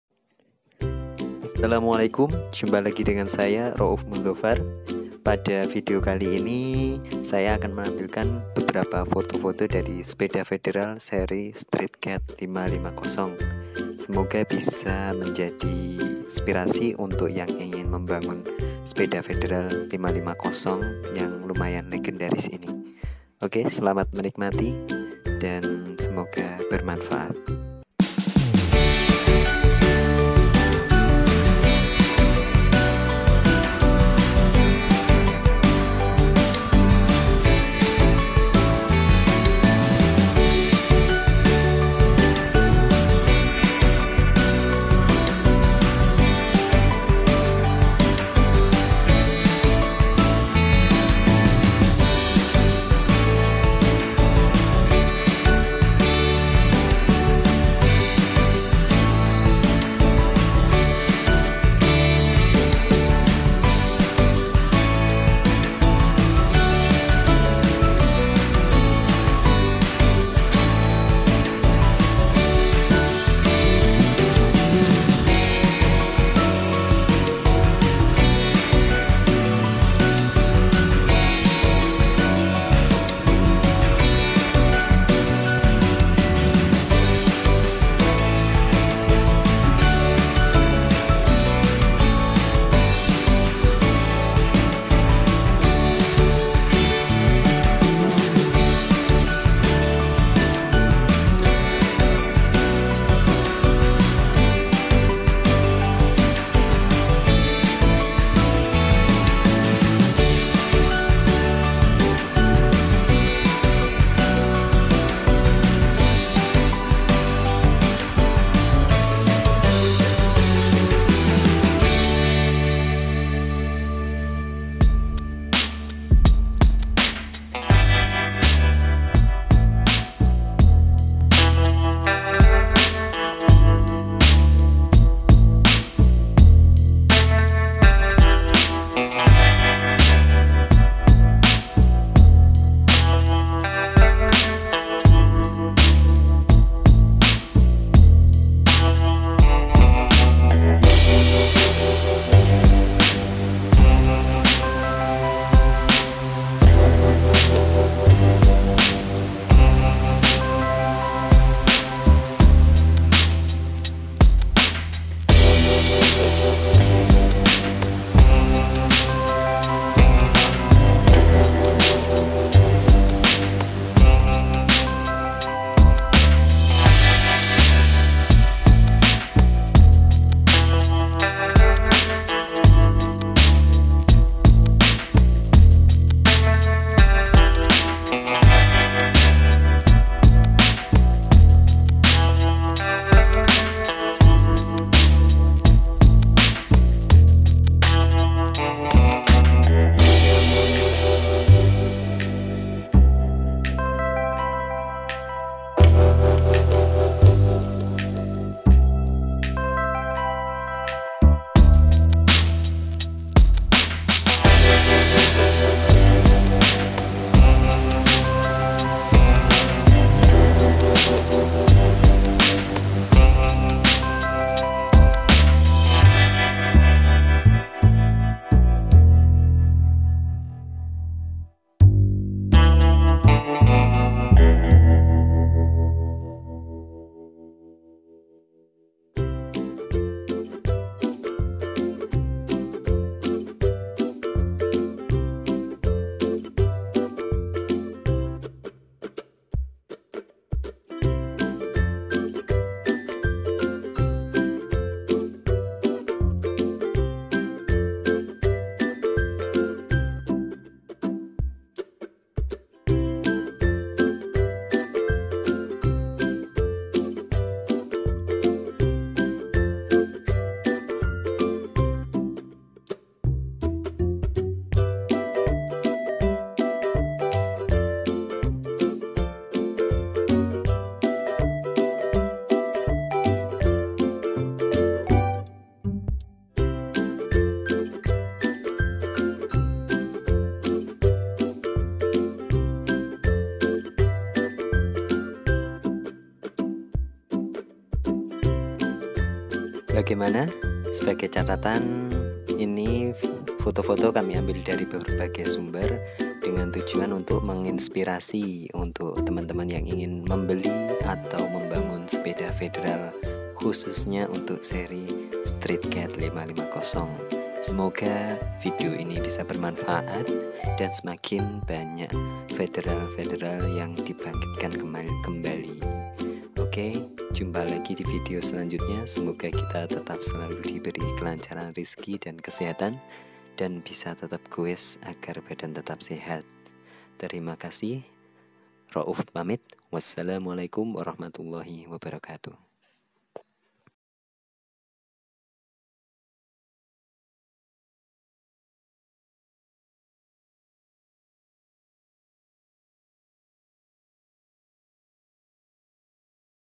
Federal 550 Street Cat Kompilasi Sepeda MTB
Pada video kali ini , kami mencoba menampilkan kompilasi sepeda MTB federal series Street Cat 550. kami ambil dari berbagai sumber dengan tujuan untuk memberikan inspirasi bagi yang ingin membeli atau membangun sepeda federal , khususnya untuk serie SC 550 ini..

semoga video ini bermanfaat dan apabila ada request masukan dan tanya jawab bisa langsung di tulis di kolom komentar

salam Federal indonesia